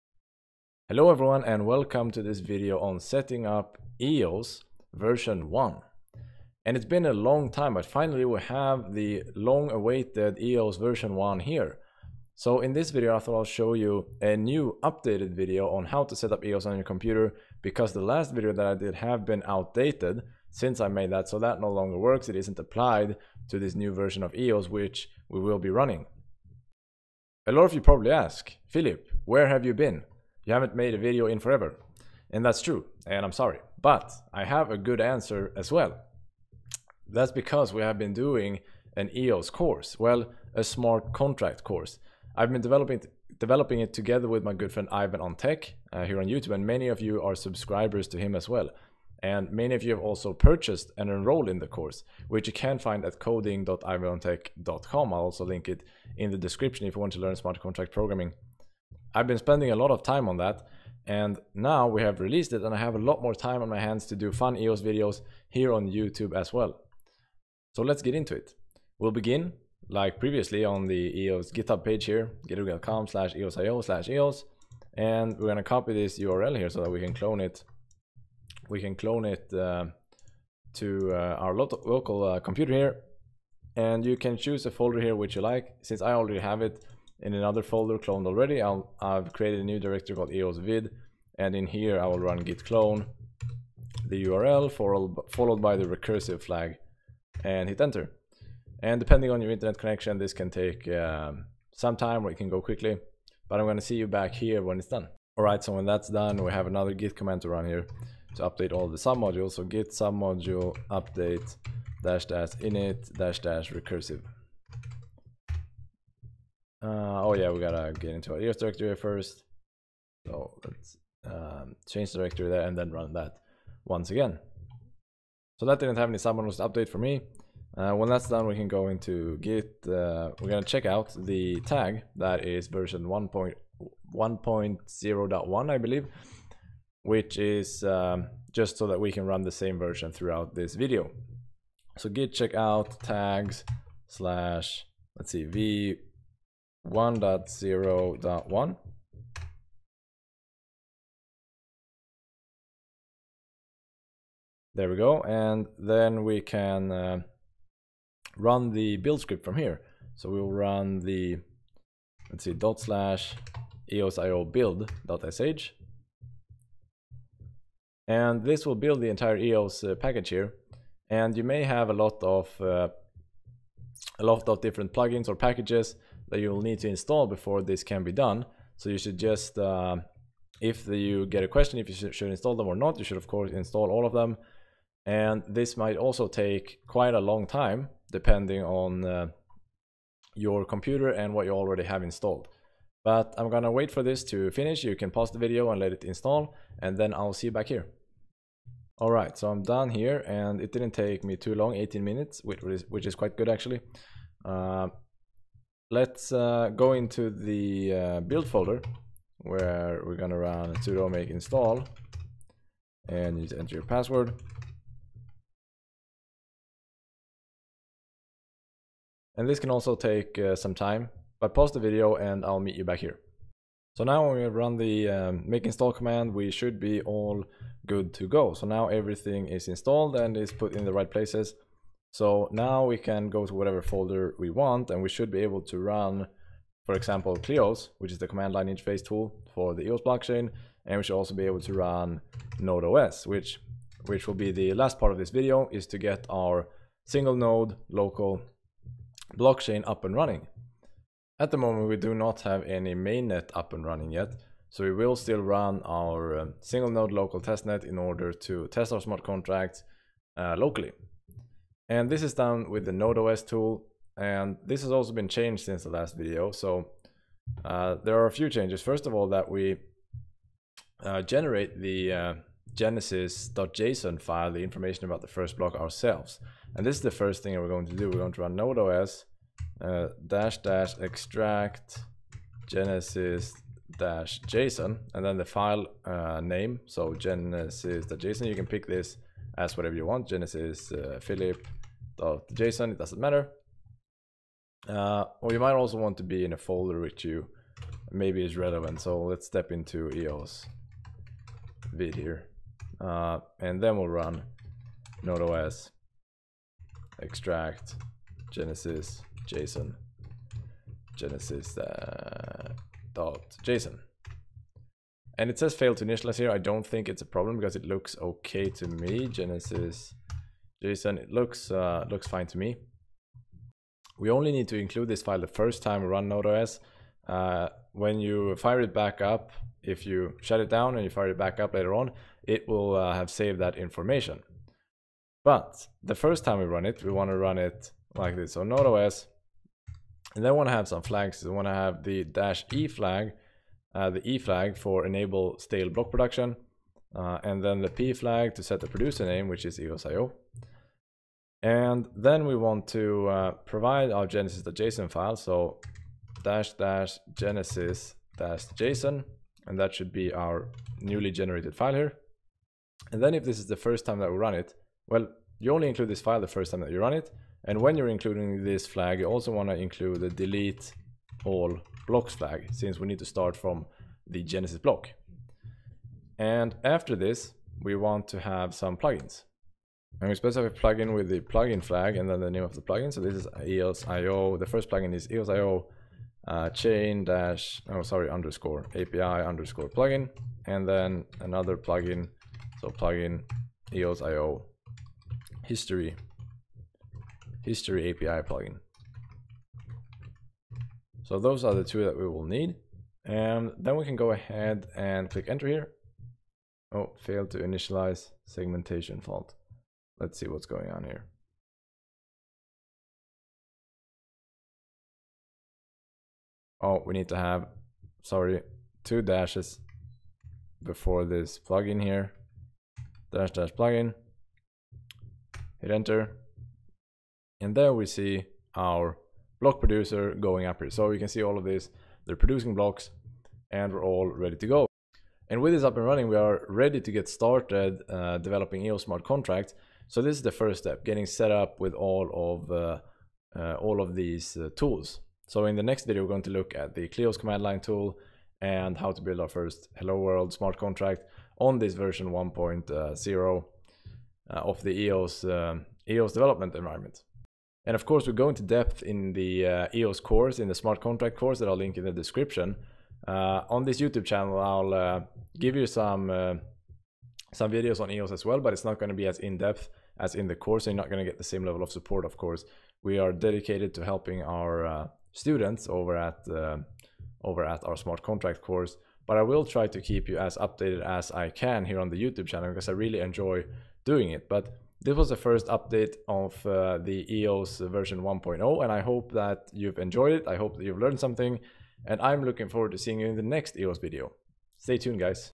EOS Programming Tutorial - Setup v1.0
Welcome to this updated video on setting up EOS, this time version 1.0. This video is meant to replace my previous video on the eos setup.

Sign up for our Smart Contract Course: coding.ivanontech.com